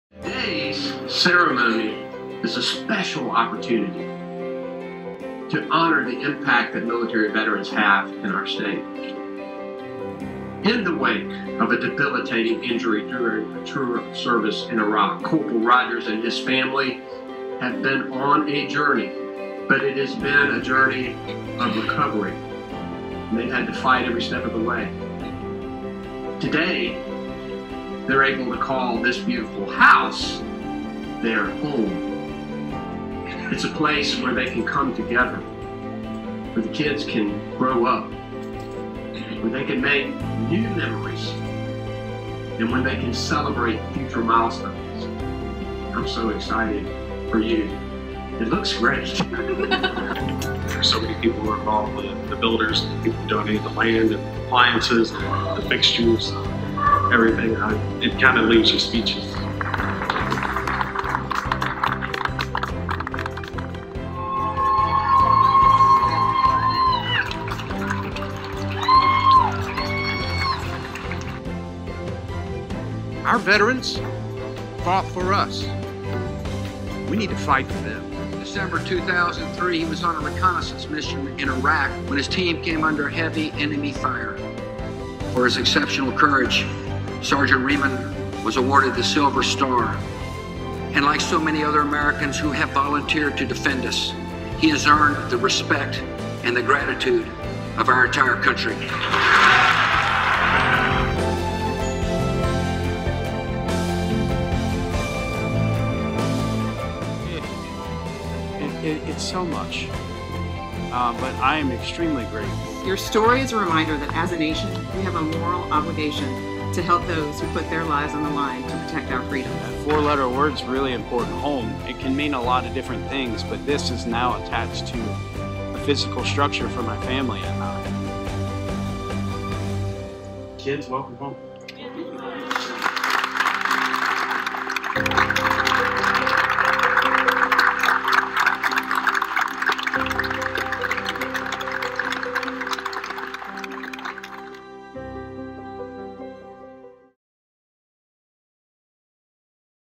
Operation Coming Home Hero Homes 18 & 19
Operation Coming Home builds homes for troops that were injured in combat operations.